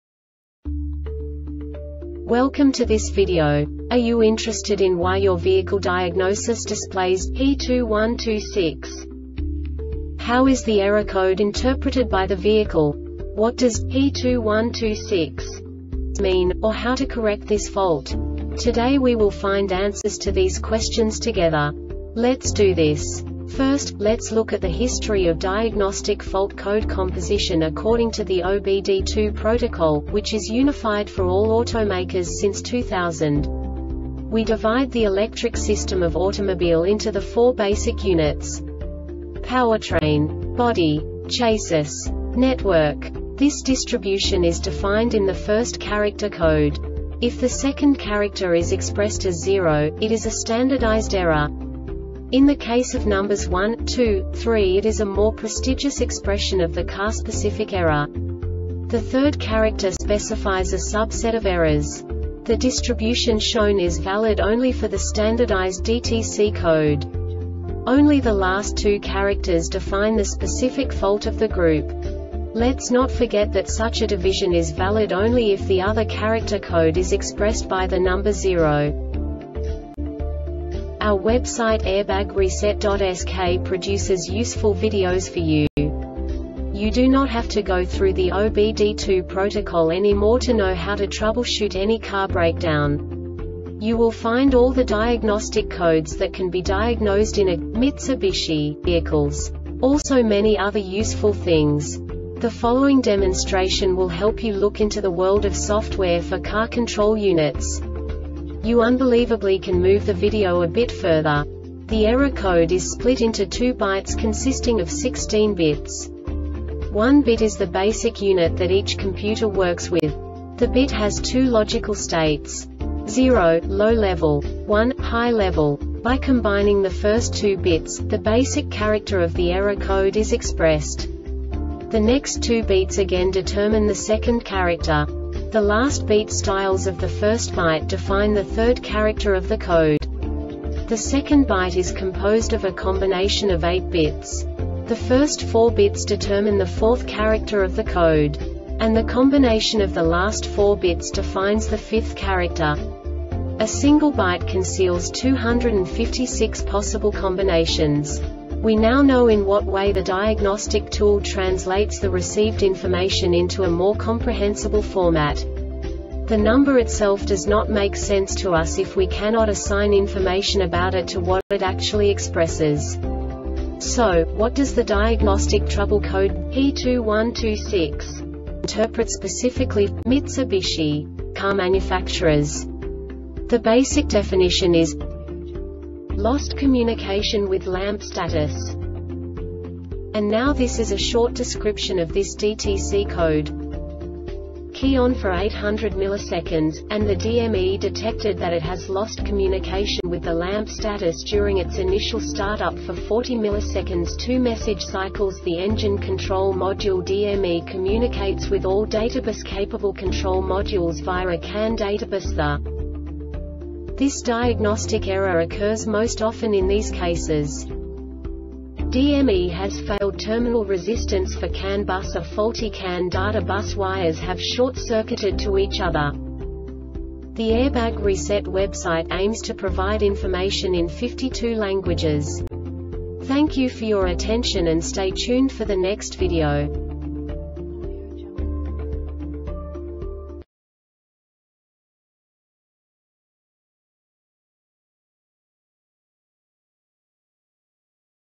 DTC Mitsubishi P2126 Short Explanation
Contents:
0:21 Basic DTC analysis according to OBD2 protocol standard.
1:48 Insight into programming
2:58 A diagnostically understandable description of the Diagnostic Trouble Code
3:05 Basic error definition

Make:
Mitsubishi
Code:
P2126
Definition:
Lost Communication With Lamp Status
Description: